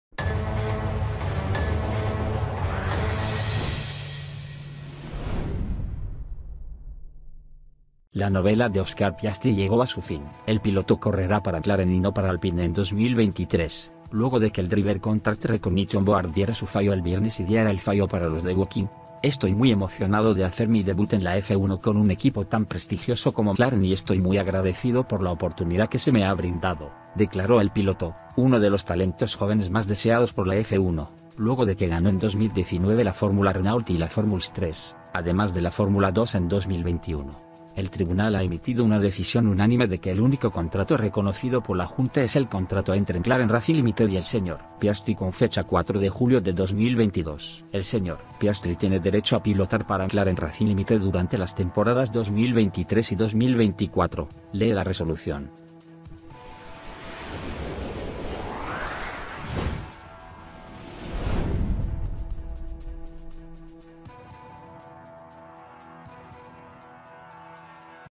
¡Es oficial! Oscar Piastri será piloto de McLaren para 2023 Alpine furioso
La novela de Oscar Piastri llegó a su fin. El piloto correrá para McLaren y no para Alpine en 2023, luego de que el Driver Contract Recognition Board diera su fallo el viernes y diera el fallo para los de Woking.

"Estoy muy emocionado de hacer mi debut en la F1 con un equipo tan prestigioso como McLaren y estoy muy agradecido por la oportunidad que se me ha brindado", declaró el piloto, uno de los talentos jóvenes más deseados por la F1, luego de que ganó en 2019 la Fórmula Renault y la Fórmuls 3, además de la Fórmula 2 en 2021.

"El Tribunal ha emitido una decisión unánime de que el único contrato reconocido por la Junta es el contrato entre McLaren Racing Limited y el Sr. Piastri con fecha 4 de julio de 2022. El Sr. Piastri tiene derecho a pilotar para McLaren Racing Limited durante las temporadas 2023 y 2024", lee la resolución.